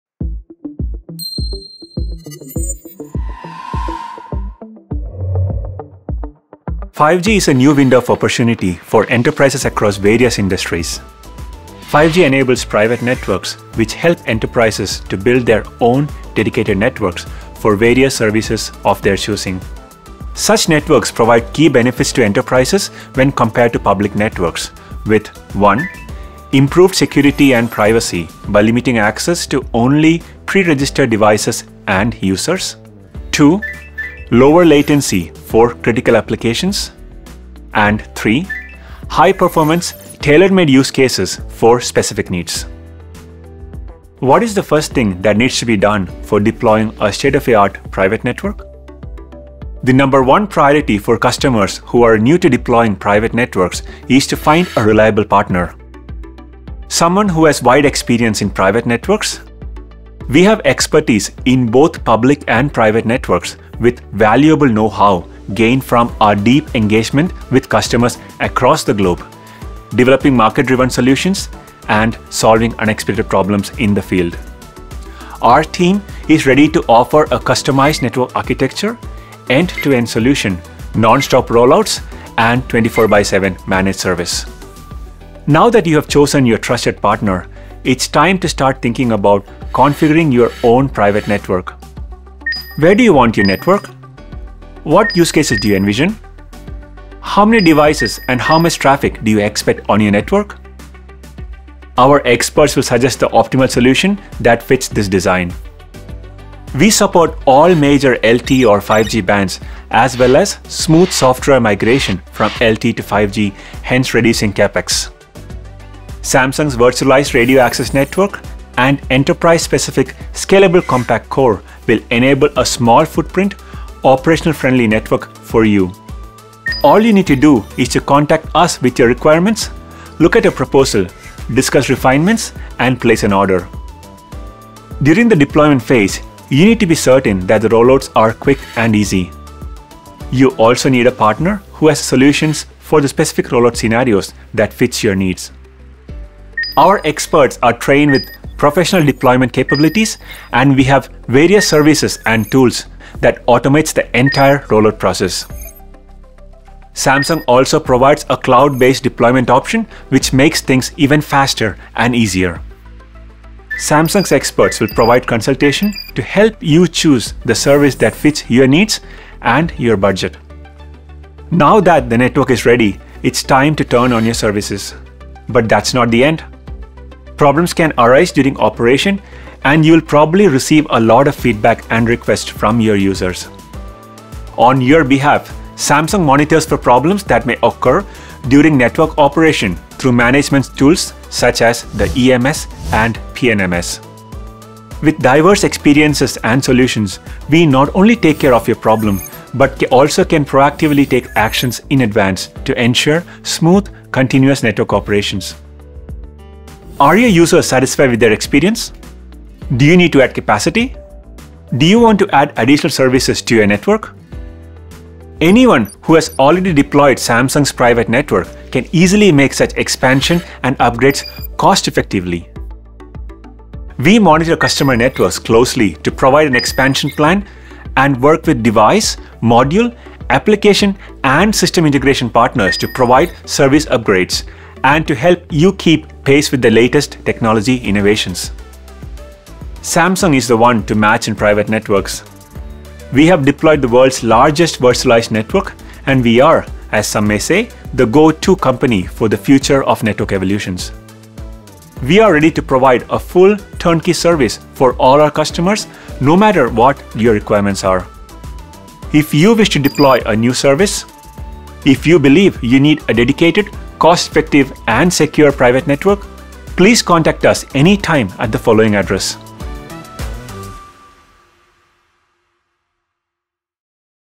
[Private Network Solutions] E2E Private Network Solutions
5G enables private networks, which helps enterprises build their own dedicated networks for various services based on their unique needs. Based on its full, turnkey solution capabilities, Samsung Networks can support enterprise customers all the way through their journey to find the best partner, design the most optimal configuration, proceed with efficient deployment and receive agile support regarding any issues or upgrade for expansion.
Samsung’s private network solution is a versatile offering that can be used for a variety of use cases.
From utility, manufacturing to smart cities, Samsung envisions many applications where private networks play a key role. Furthermore, with the rising need of safety and public services in times of emergencies from nations across the world, Samsung offers an end-to-end public safety solution. We have already supported nationwide deployment and commercialization in multiple countries. We are also leading the 3GPP industry standards and will continue to pioneer the public safety market.